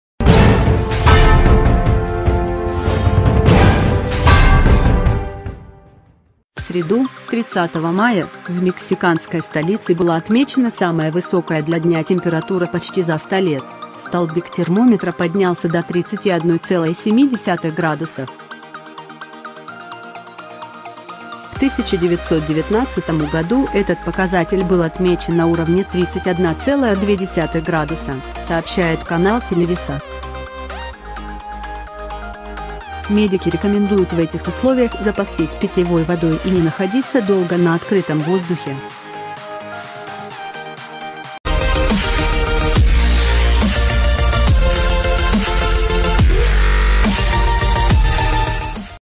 В Мексике объявили режим ЧП из-за аномальной жары
Всреду, 30 мая, в мексиканской столице была отмечена самая высокая для дня температура почти за 100 лет – столбик термометра поднялся до 31,7 градусов.В 1919 году этот показатель был отмечен на уровне 31,2 градуса, сообщает канал Televisa.Медики рекомендуют в этих условиях запастись питьевой водой и не находиться долго на открытом воздухе.